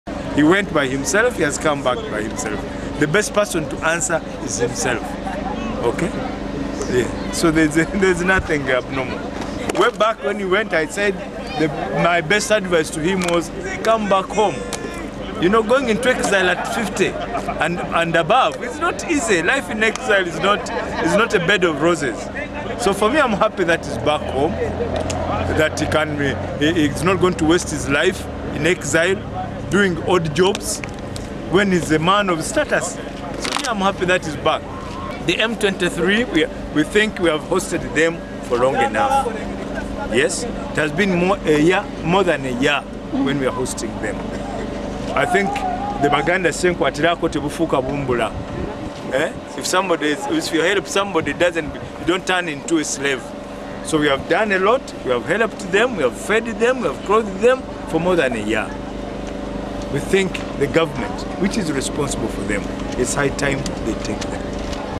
Gen Katumba Wamala says Sejusa could not have managed life in exile
The Chief of Defense Forces General Edward Katumba Wamala has said that the harsh life in exile forced General David Sejusa to return to Uganda. Sejusa, the former coordinator of intelligence services, fled to the United Kingdom in May last year. Katumba Wamala told journalists in Mbarara that life abroad is not a bed of roses.  He also spoke on the M23 rebels who recently fled Bihanga Military camp in Ibanda district to avoid being repatriated to DRC. Gen. Katumba Wamala was speaking at the closure of the UPDF’s football tournament.